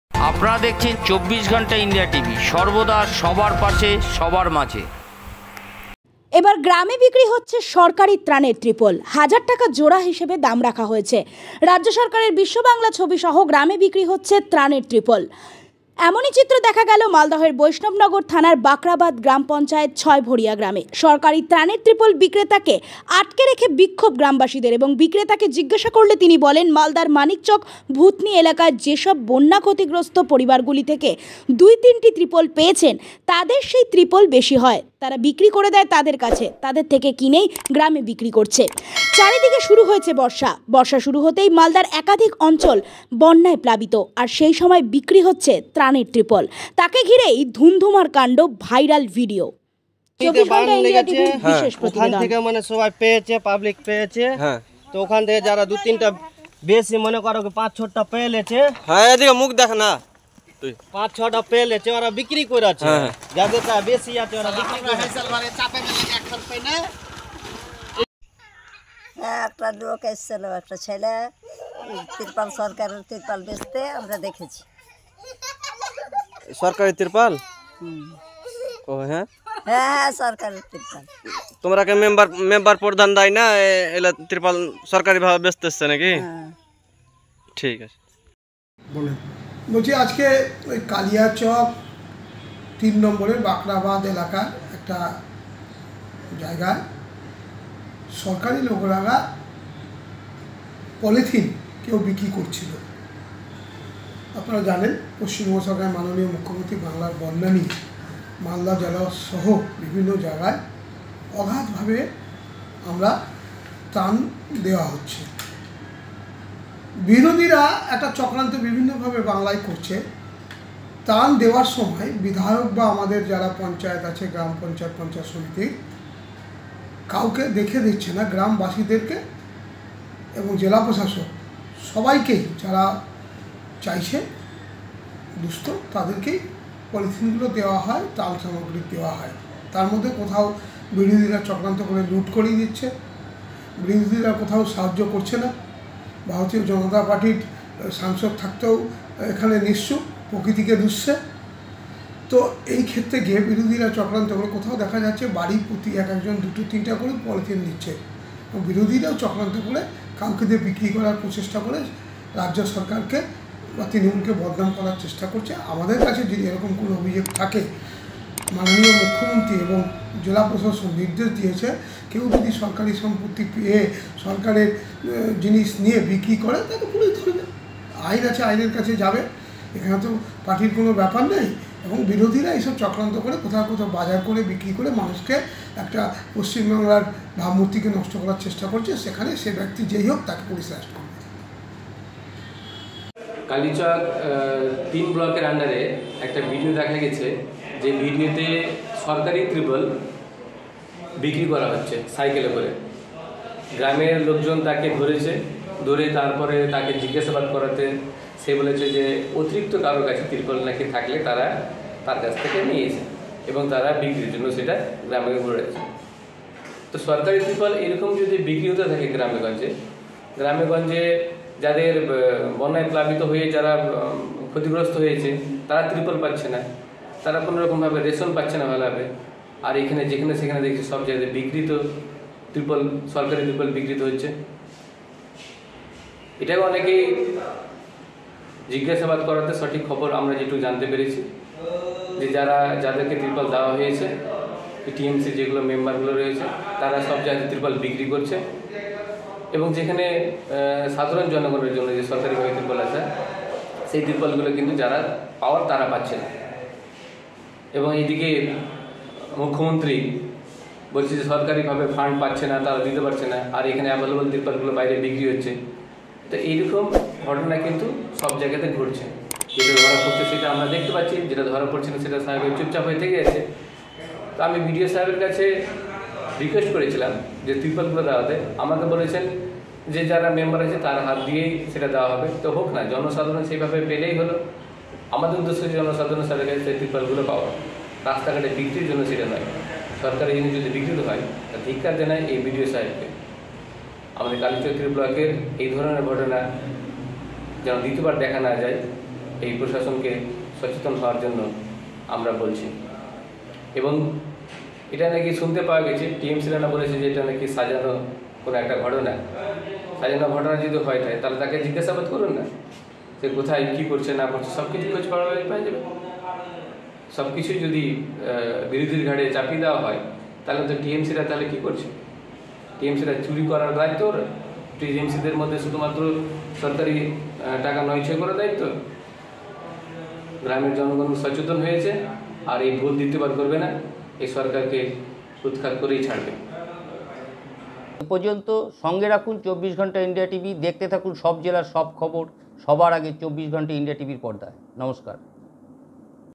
24ghantaindiatvমালদা;এবার গ্রামে বিক্রি হচ্ছে সরকারি ত্রাণের ত্রিপল হাজার টাকা জোড়া
24ghantaindiatvমালদা;এবার গ্রামে বিক্রি হচ্ছে সরকারি ত্রাণের ত্রিপল হাজার টাকা জোড়া হিসেবে দাম রাখা হয়েছে। রাজ্য সরকারের বিশ্ব বাংলা ছবি সহ গ্রামে বিক্রি হচ্ছে প্রাণের ত্রিপল। এমনই চিত্র দেখা গেল মালদাহের বৈষ্ণবনগর থানার বাখরাবাদ গ্রাম পঞ্চায়েতের ছয় ভরিয়া গ্রামে। সরকারি ত্রাণের ত্রিপল বিক্রেতা কে আটকে রেখে বিক্ষোভ গ্রামবাসীদের এবং বিক্রেতাকে জিজ্ঞাসা করলে তিনি বলেন মালদার মানিকচক ভূতনি এলাকায় যেসব বন্যা ক্ষতিগ্রস্ত পরিবার গুলি দুই থেকে তিনটি ত্রিপল পেয়েছেন তাদের সেই ত্রিপল বেশি হয়, তারা বিক্রি করে দেয় তাদেরই কাছ থেকে কিনে এনে গ্রামে বিক্রি করছি। চারিদিকে শুরু হয়েছে বর্ষা আর বর্ষা শুরু হতেই মালদার একাধিক অঞ্চল বন্যায় প্লাবিত আর সেই সময় বিক্রি হচ্ছে ত্রাণের ত্রিপল তাকেই ঘিরে ধুমধুমর কান্ড ভাইরাল ভিডিও।